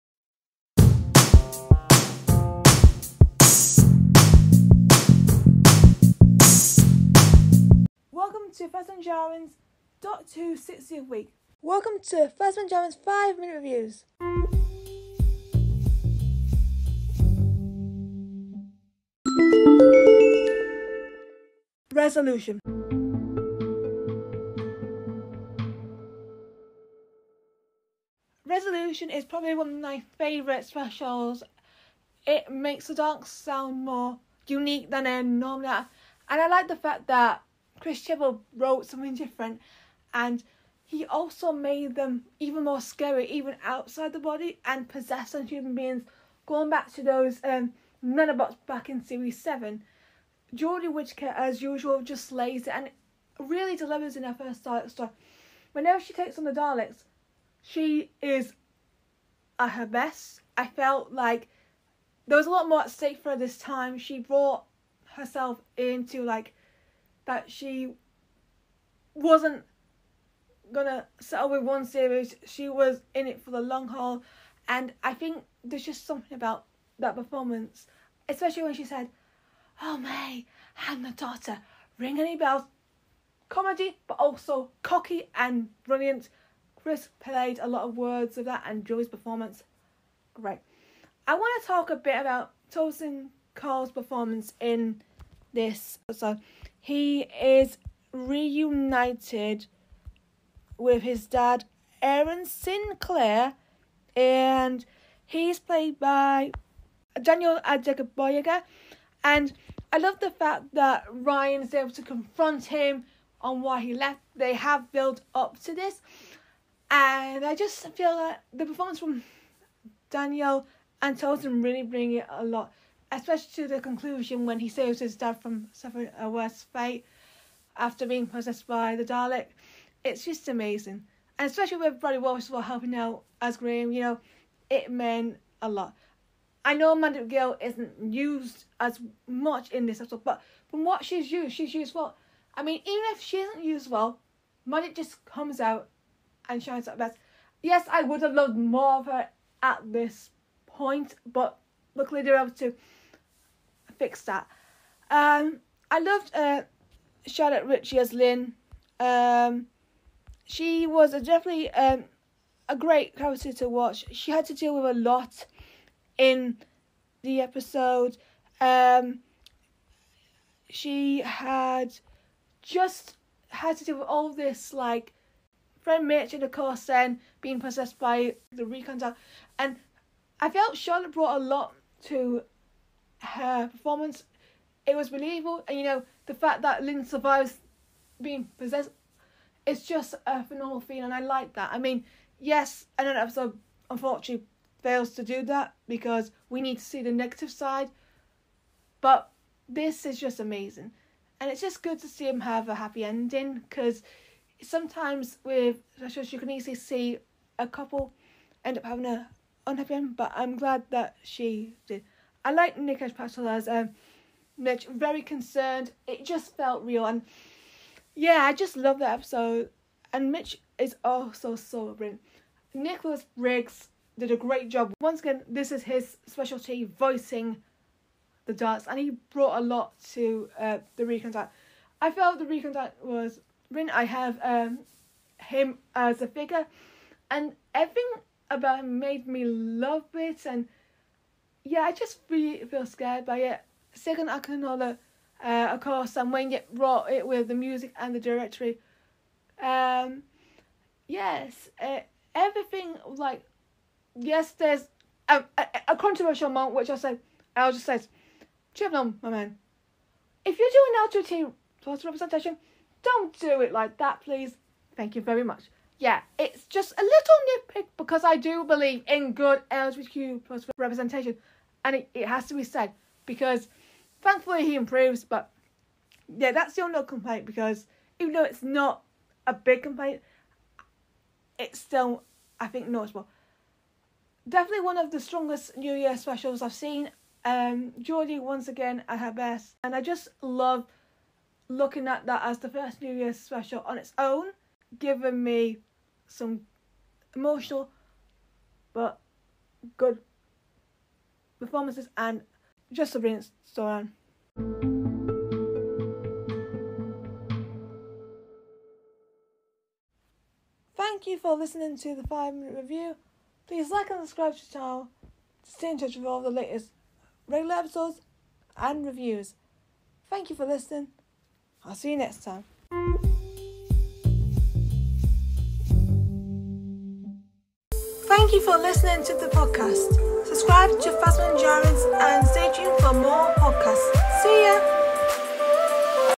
Jodie Whittaker slays FIRST Dalek story - Resolution Review
I review Jodie Whittaker’s FIRST Dalek story.